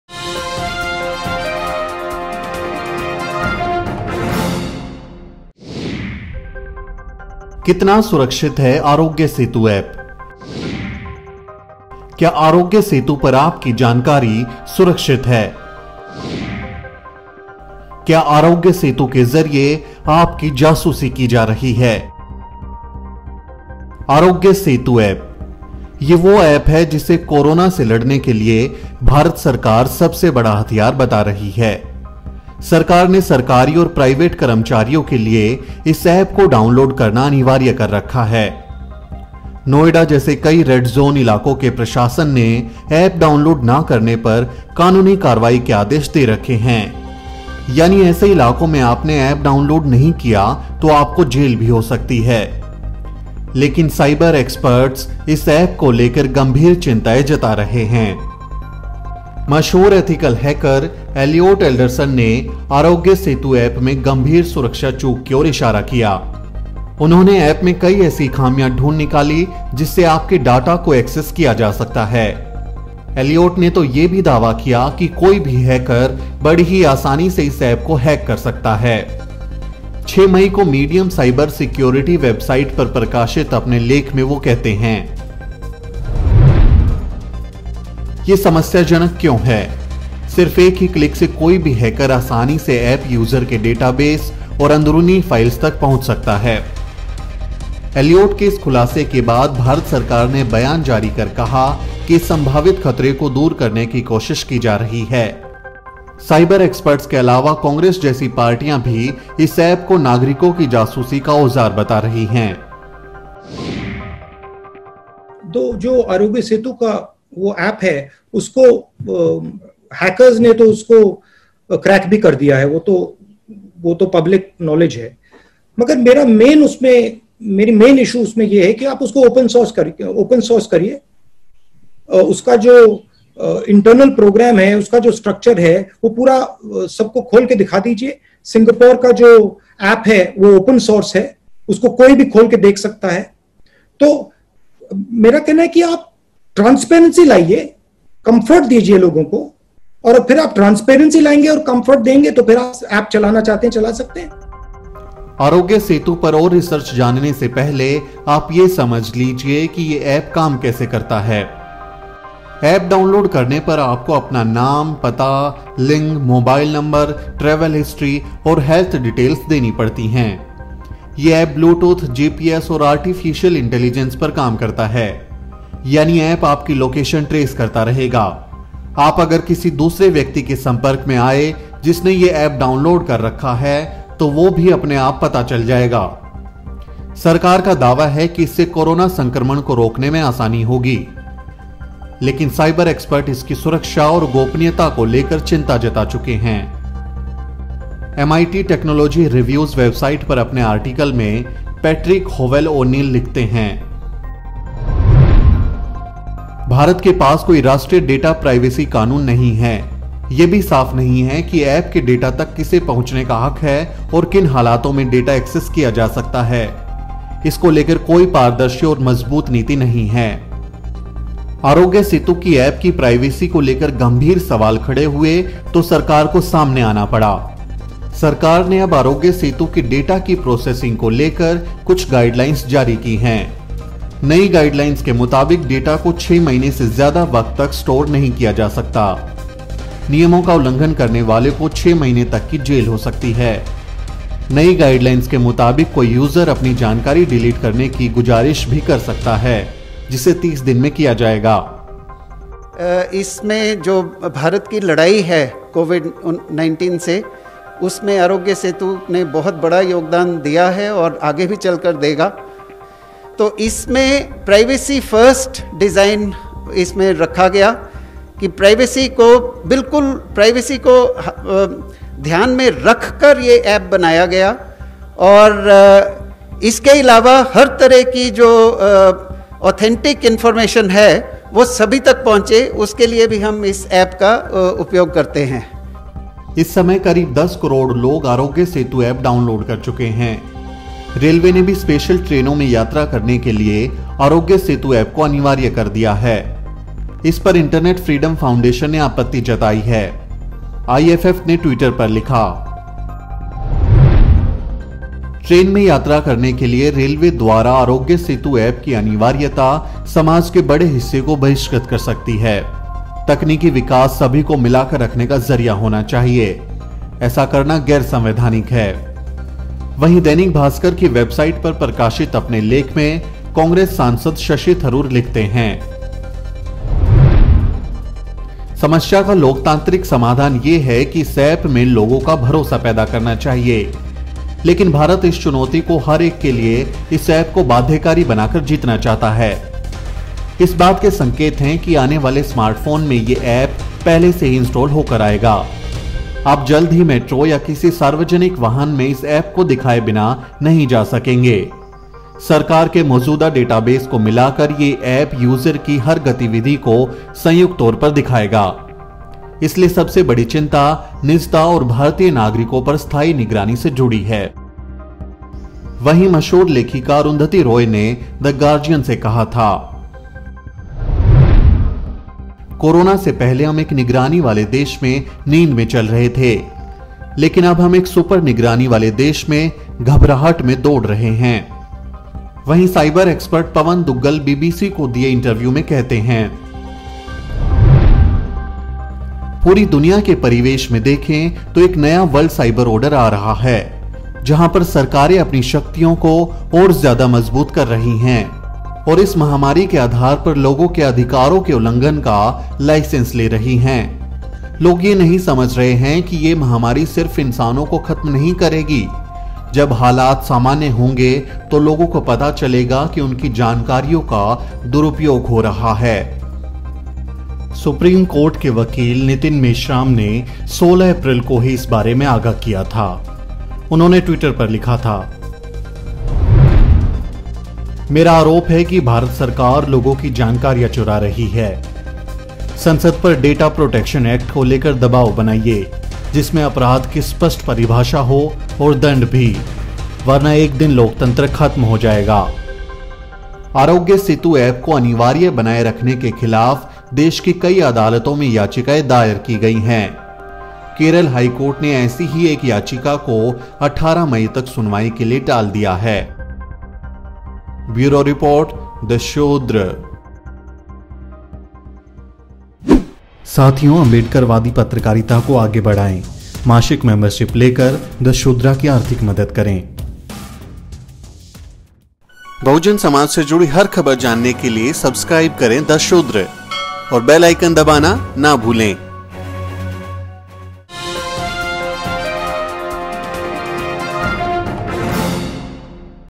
कितना सुरक्षित है आरोग्य सेतु ऐप? Aarogya Setu Mobile App Explained | Data Privacy | Elliot Alderson
क्या आरोग्य सेतु पर आपकी जानकारी सुरक्षित है? क्या आरोग्य सेतु के जरिए आपकी जासूसी की जा रही है ?

Video/Youtube/Ministry of AYUSH
Video/Ministry of Railways
Video/Twitter/@fs0c131y